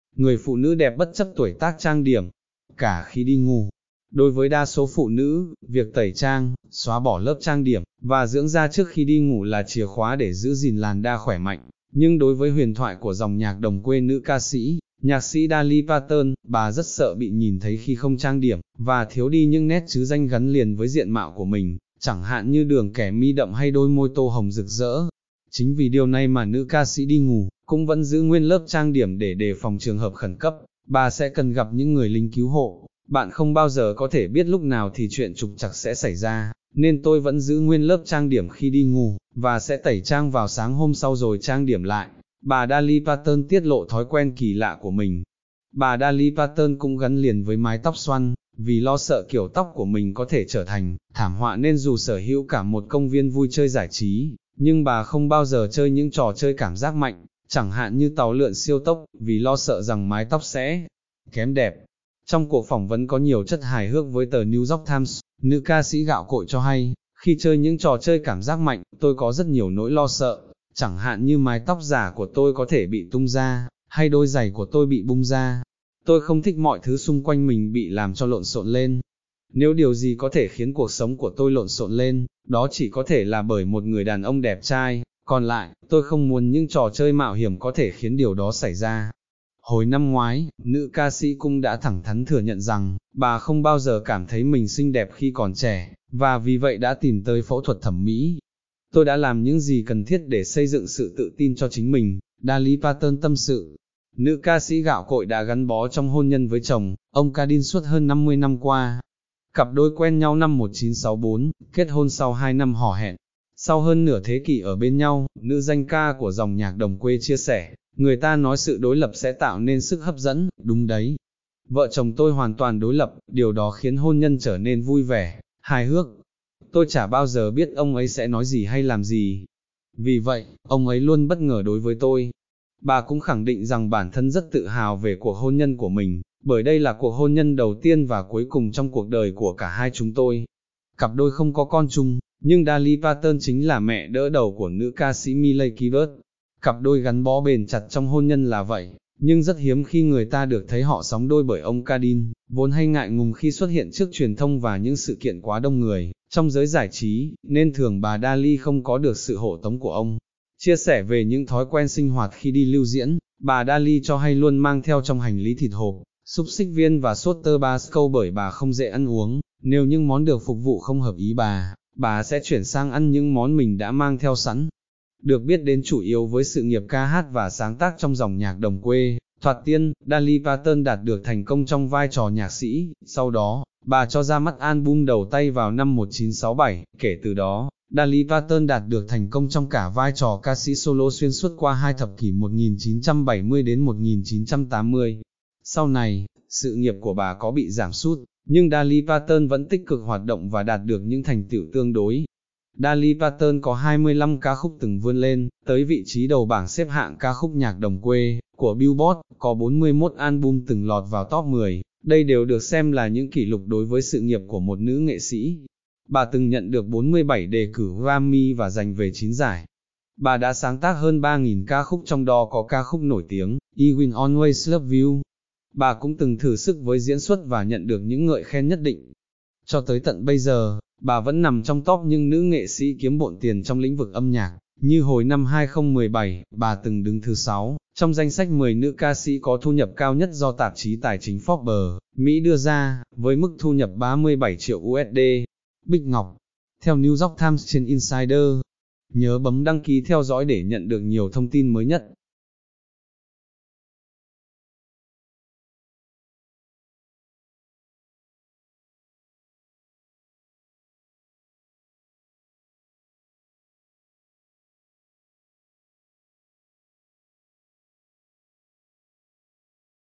“Người phụ nữ đẹp bất chấp tuổi tác” trang điểm… cả khi đi ngủ
(Dân trí) - Nữ ca sĩ người Mỹ Dolly Parton (73 tuổi) - huyền thoại của dòng nhạc đồng quê - vừa tiết lộ rằng bà trang điểm cả khi đi ngủ để đề phòng trường hợp xảy ra sự cố, chẳng hạn như... hỏa hoạn, sẽ có những tốp lính cứu hỏa ập vào giải cứu bà.